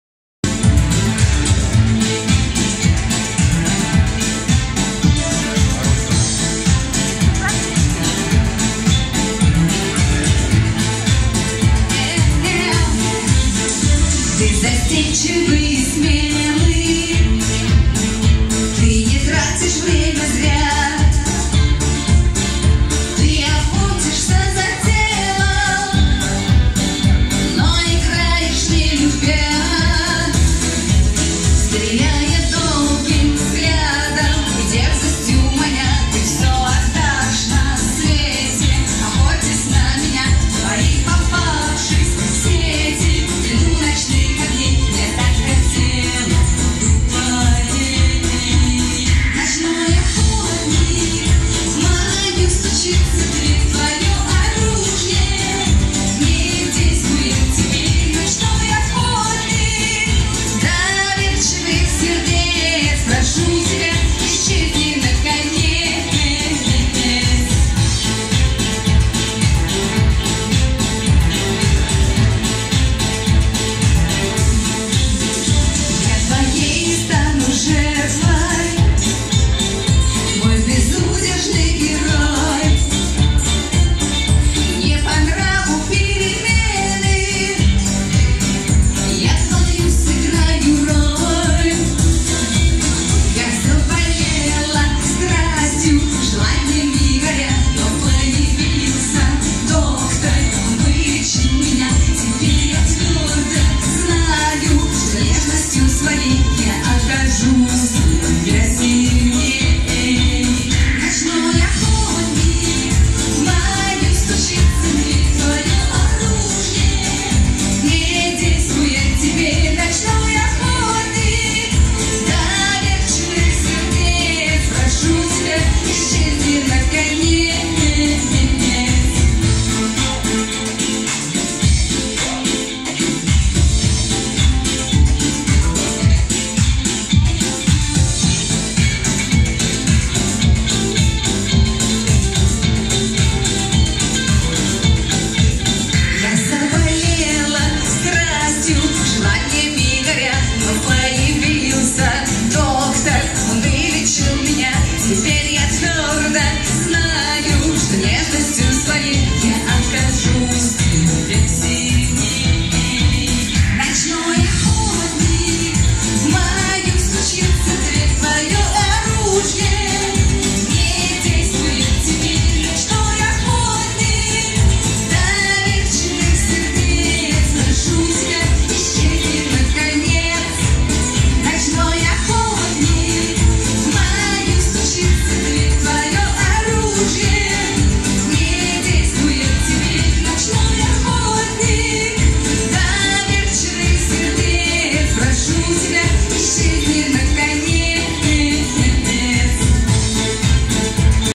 Светлана Разина
СВЕТЛАНА РАЗИНА, первая солистка группы "МИРАЖ" в ресторане "Гладиатор". Концерт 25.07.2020 г.